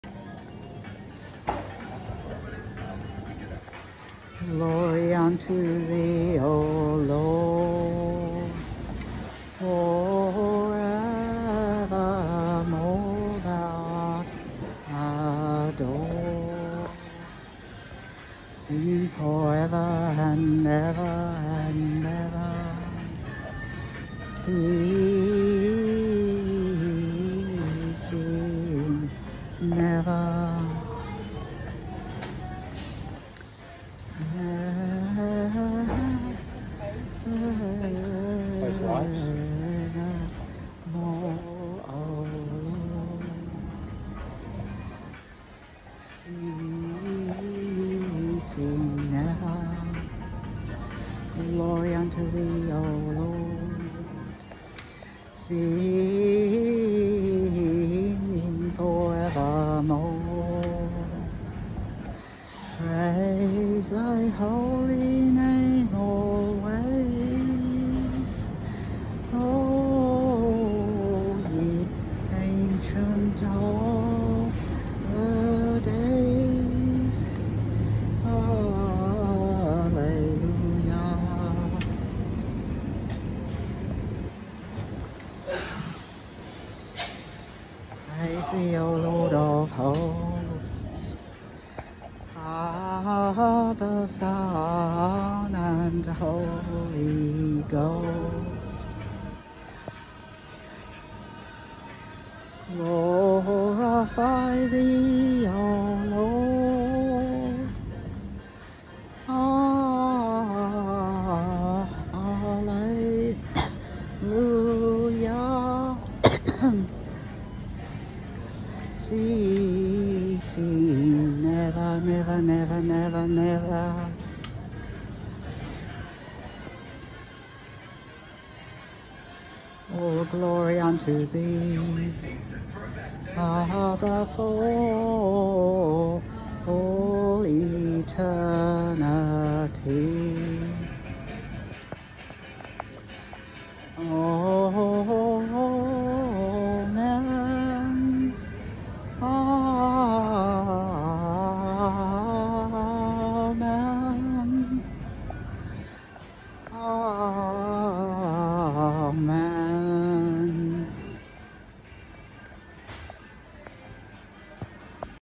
ETERNAL PRAISE & GLORY 22nd August 2024
🎵Eternal Praise and Glory 🎶
Every Step A Prayer, Every Breath A Psalm.

    O DREAD-SOVEREIGN LORD,
    My LOVING SAVIOUR GOD.
I Praise and Glorify Thee forevermore! O My LORD.
I Praise Thee forever in Thy sanctuary:
I Praise Thee without ceasing, in the firmament of Thy power.
I Praise Thee eternally, for Thy mighty acts:
I Praise Thee joyfully, according to Thy excellent greatness.
I Praise Thee zealously, with the sound of the trumpet:
I Praise Thee silently, with the psaltery and harp.
I Praise Thee victoriously, with the timbrel and dance:
I Praise Thee humbly, with stringed instruments and organs.
I Praise Thee continually, upon the loud cymbals:
I Praise Thee courageously, upon the high sounding cymbals.
Let all Thy Creation, and every thing that hath breath praise and honour Thee, O LORD.
Praise ye the LORD.

      BY GOD’S GRACE ABOUNDING.

Psalm 150

Recored in Supermarket